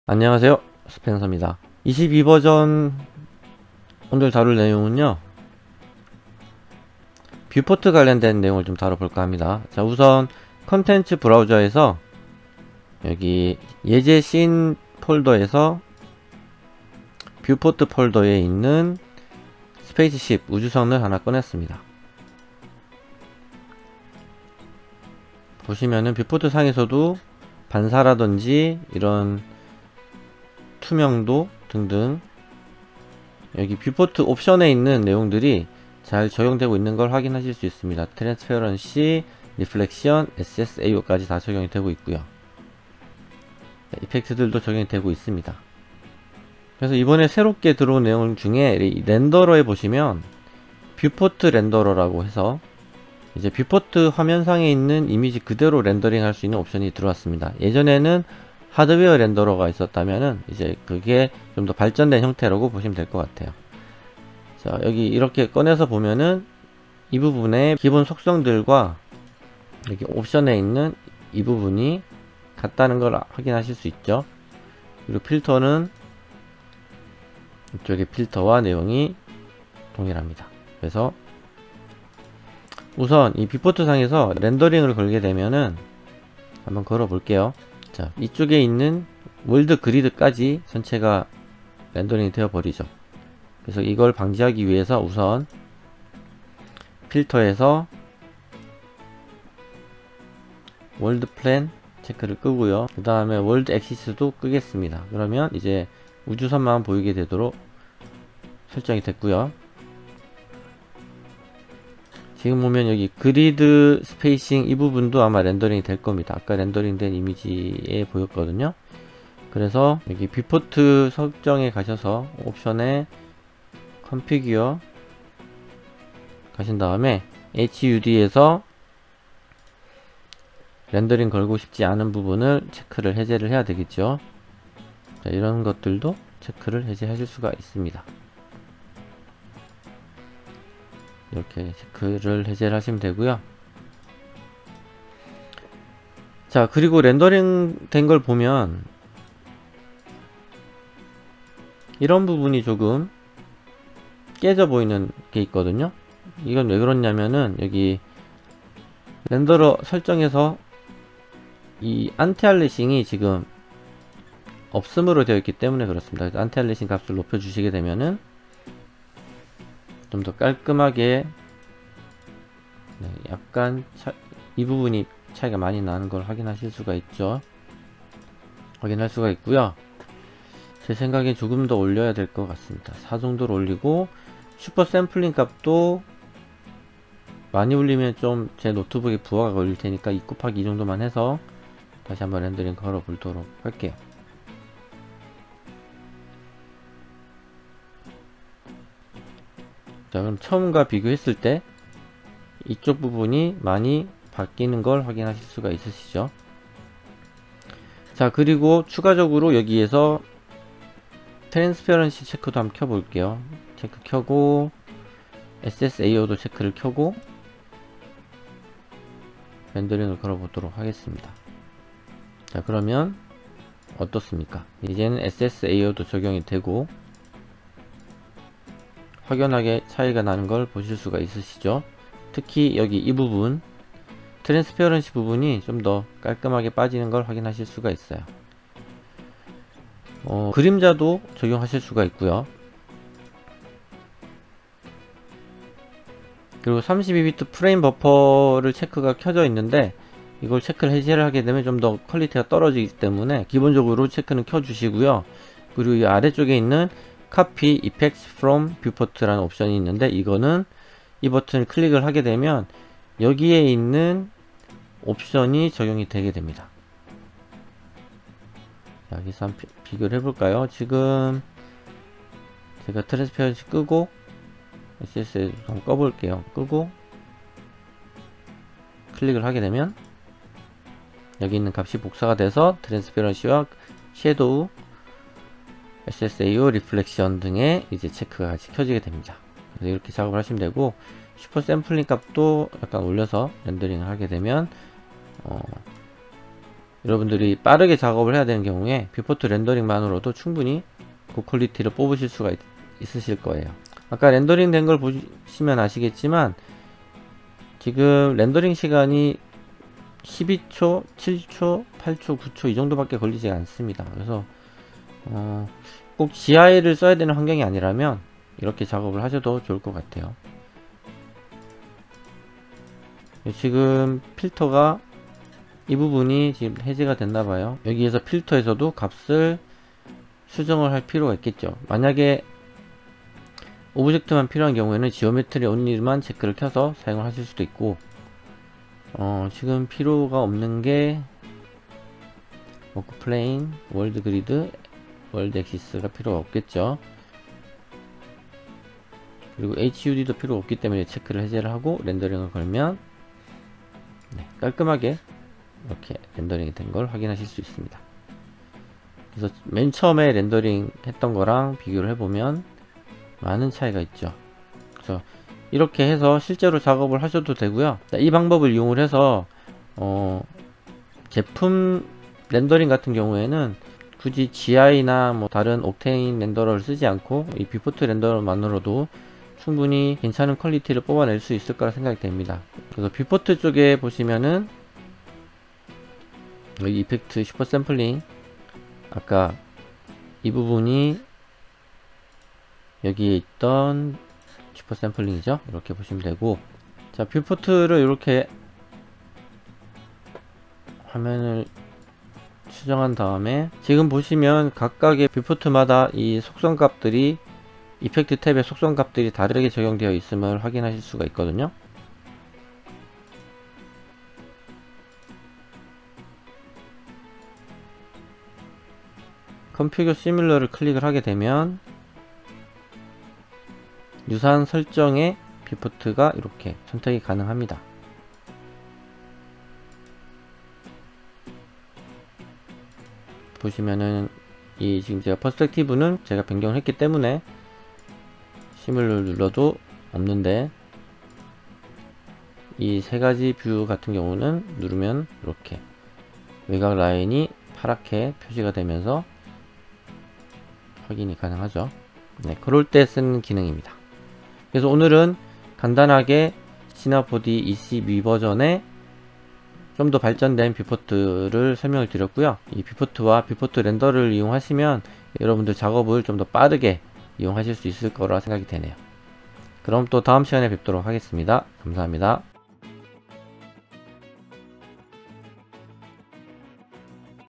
Cinema 4D S22 Viewport and Viewport Renderer(subtitle english, korean, japanese)
Introducing S22 Viewport and Viewport Renderer
Let's see how to use the new Viewport and Viewport Renderer in Cinema 4D S22.

Cinema 4D S22에서 새롭게 바뀐 Viewport와 Viewport Renderer 사용 방법에 대해 알아보겠습니다.